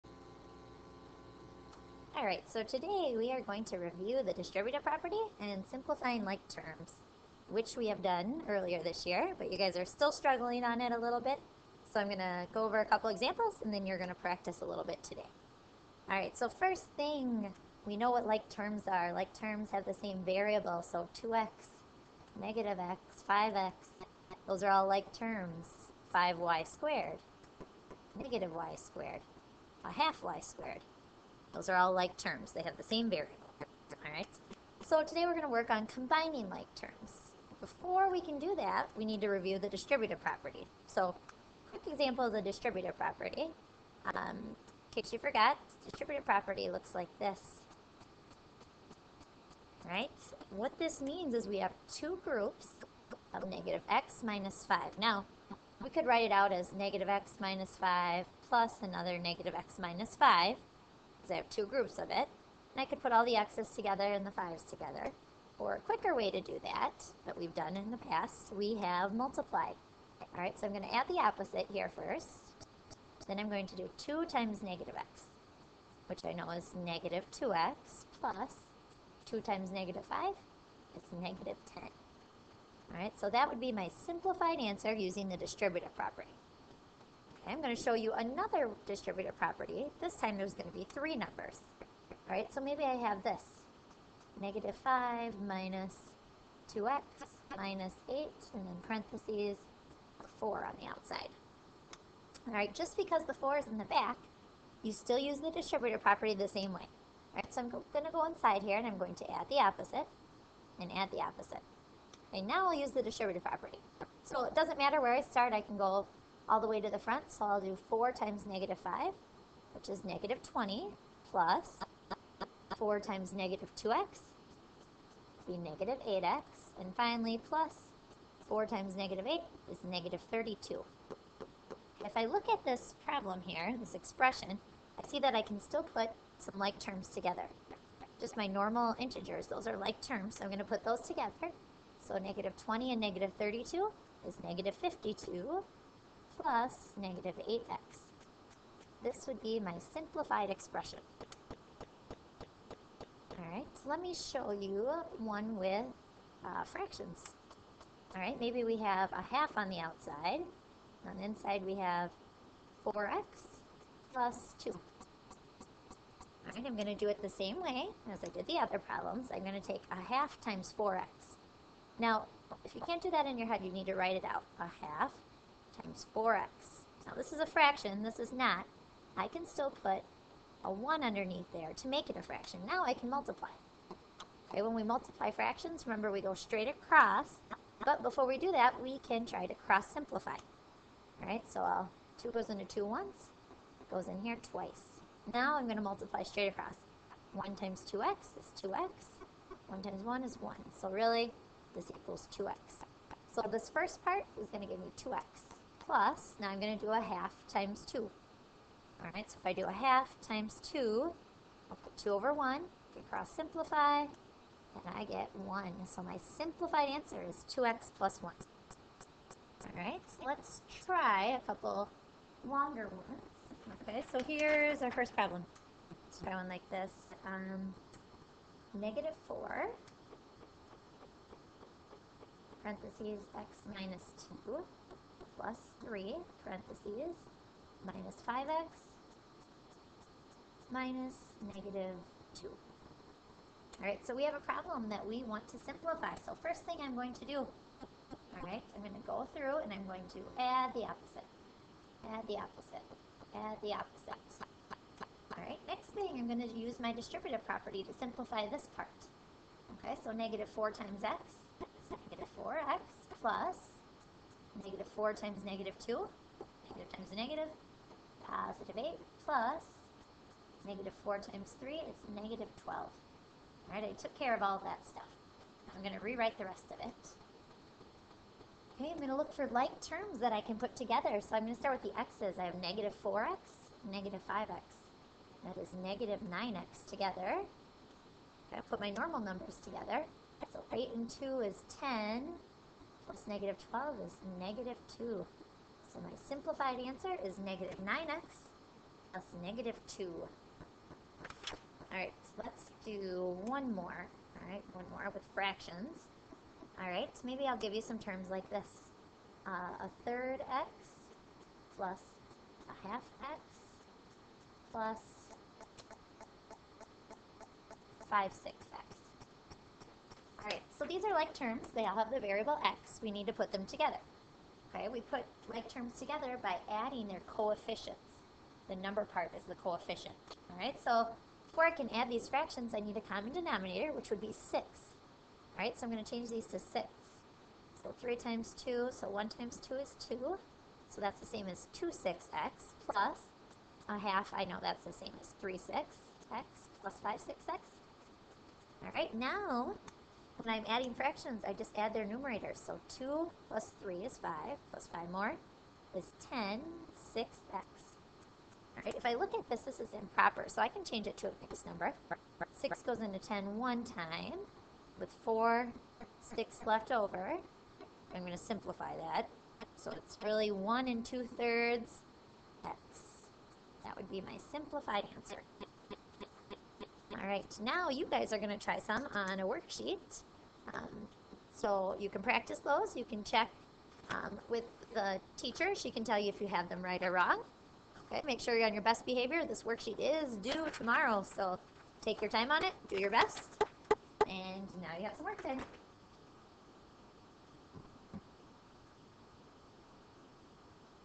Accelerated- Simplifying & Distributive Property.wmv
A short video that reviews the distributive property and simplifying expressions with the distributive property. This video also has examples with fractions.